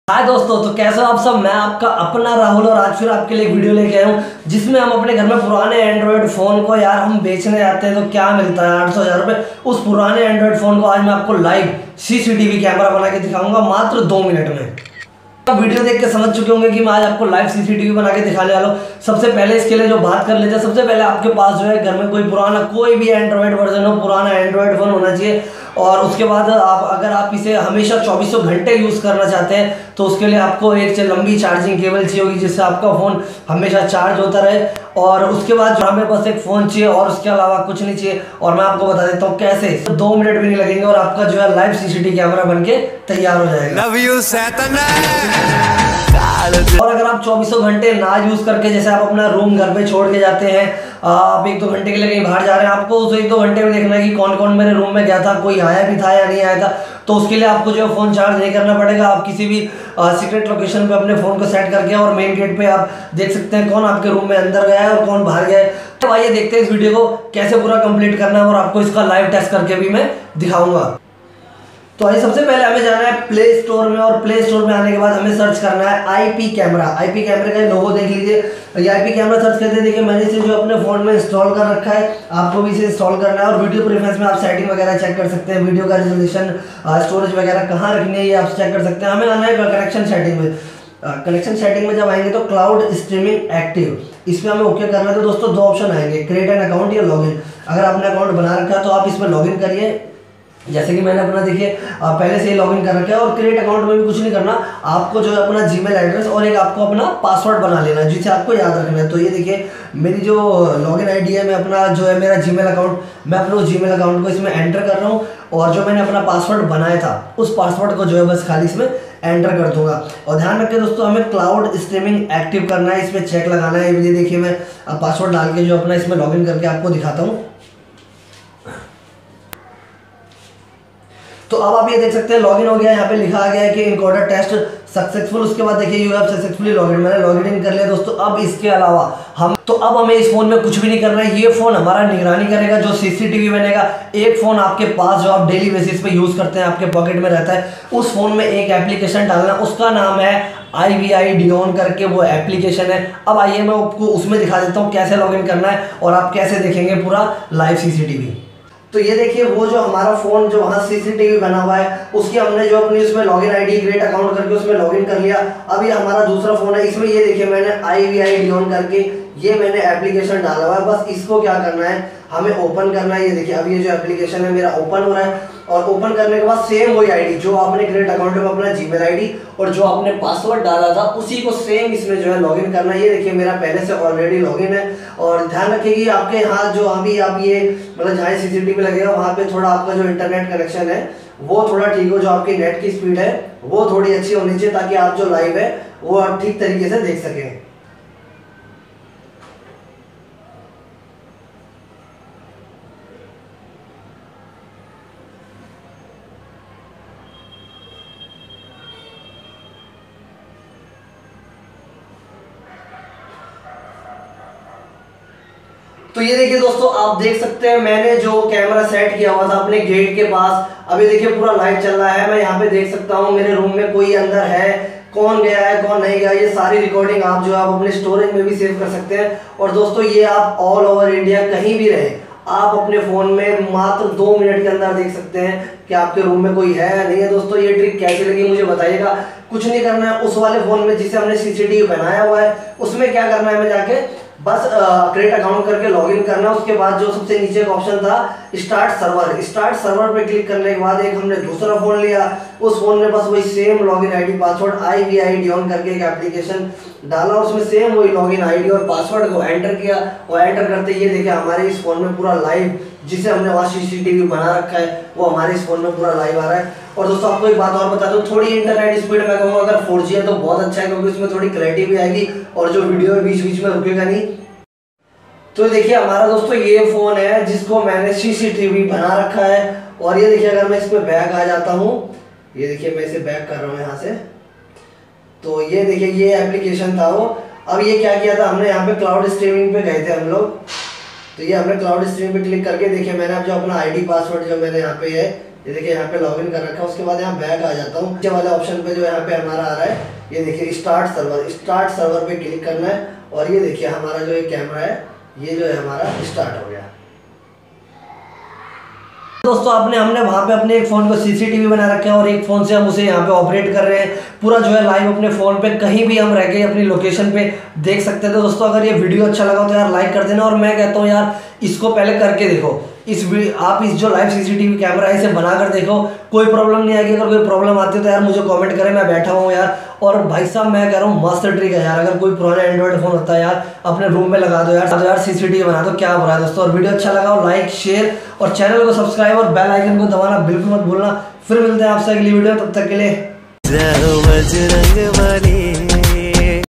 How To Make CCTV Of Your Mobile || पुराने मोबाइल को CCTV कैसे बनाये at Home
Dosto Aaj Btane Wala hu Kaise Apne Old Android Mobile Ko cctv Camera Banaye Aor Kahi se Bhi Live Dekhe.
Bas 2 Min No Bakwass Sirf Live Baat.

Any Prblm Plz Cmmnt Plz Support.